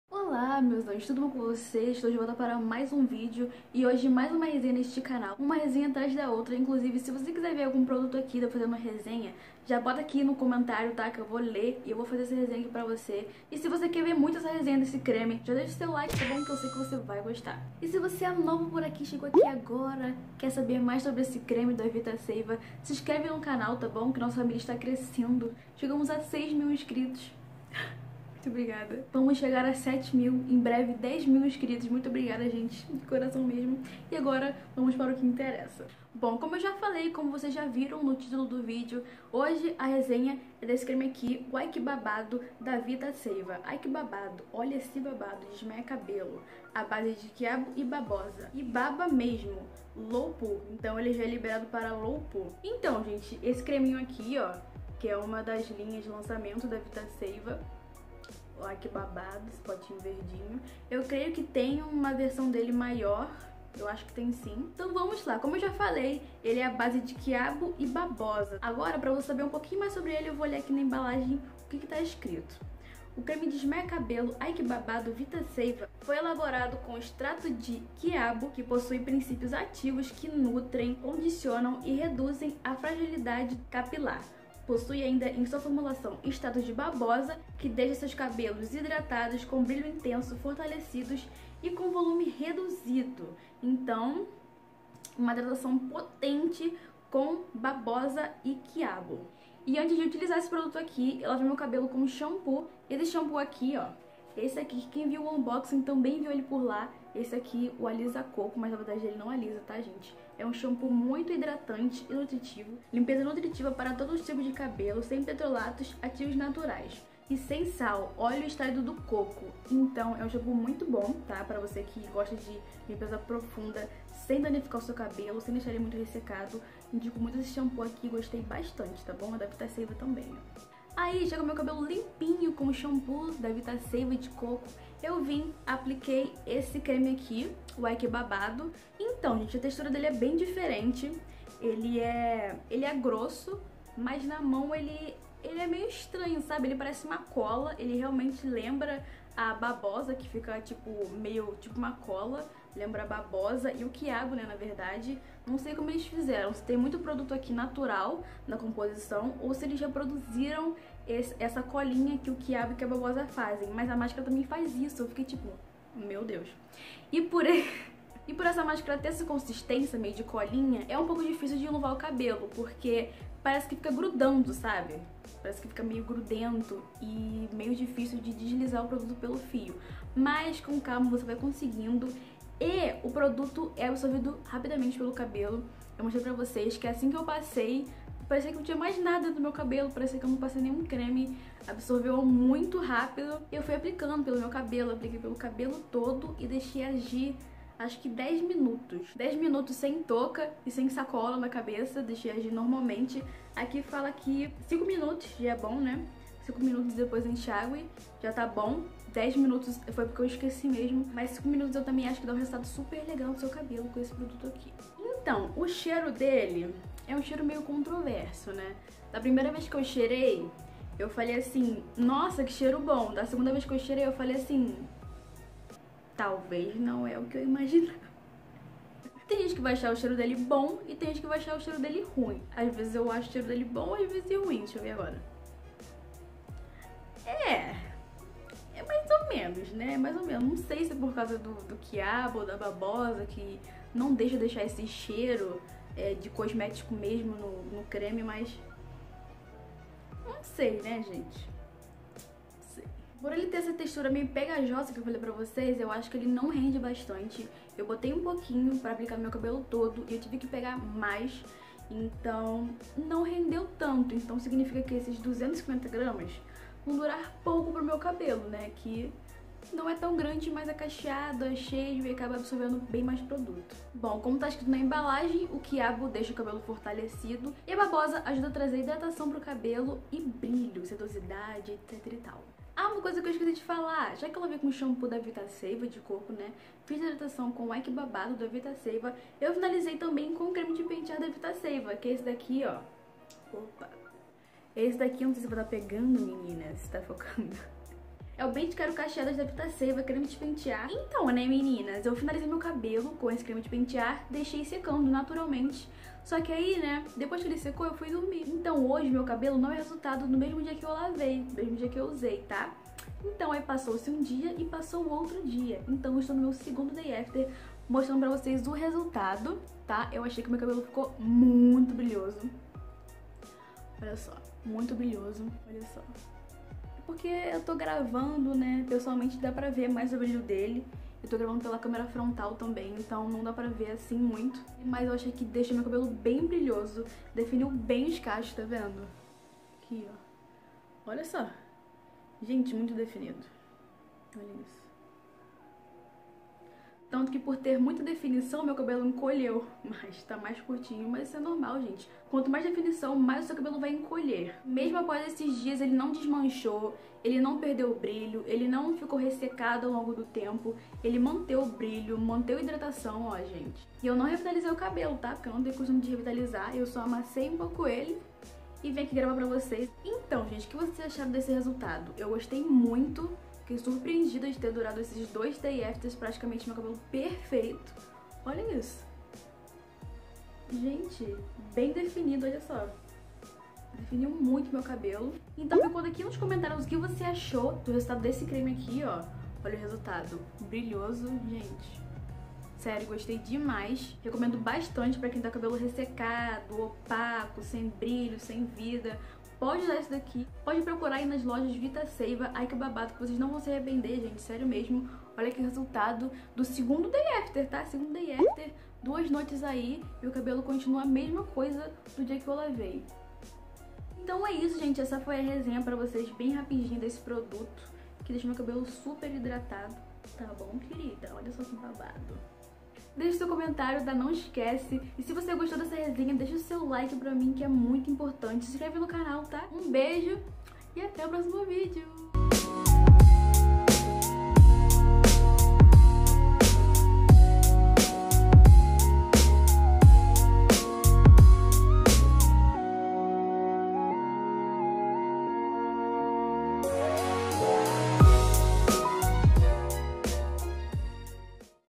Vita Seiva - AI QUE BABADO Desmaia cabelo • cachos perfeitos durante 2 days afters | resenha
Olá meus amoresss!
Hoje trago mais uma resenha pra vocês desse produto que me surpreendeu muito!
O AI QUE BABADO da Vita Seiva, um creme de tratamento de hidratação a base de quiabo e babosa.